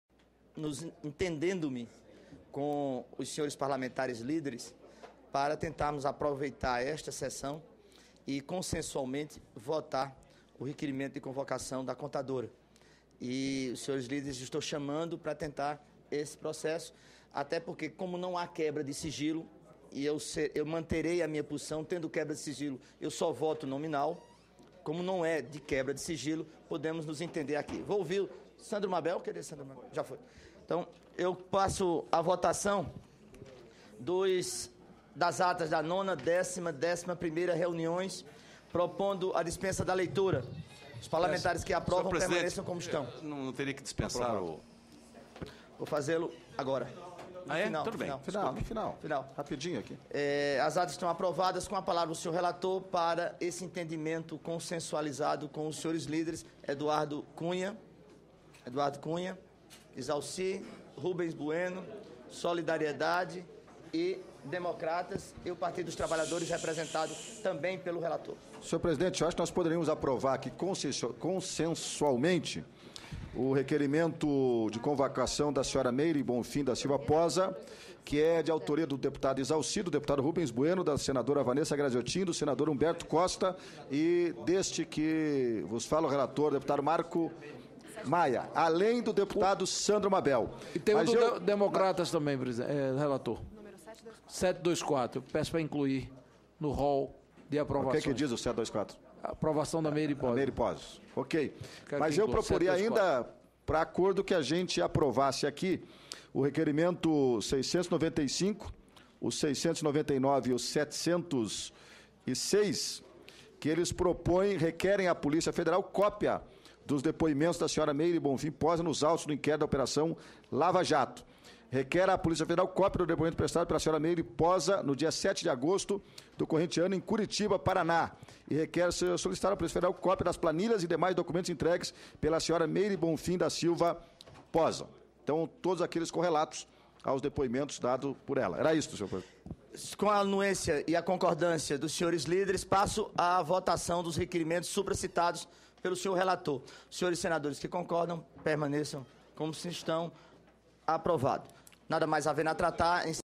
CPMI aprova convocação de Meire Poza, ex-contadora de Alberto Youssef
17/09/2014 - CPMI - CPMI aprova convocação de Meire Poza, ex-contadora de Alberto Youssef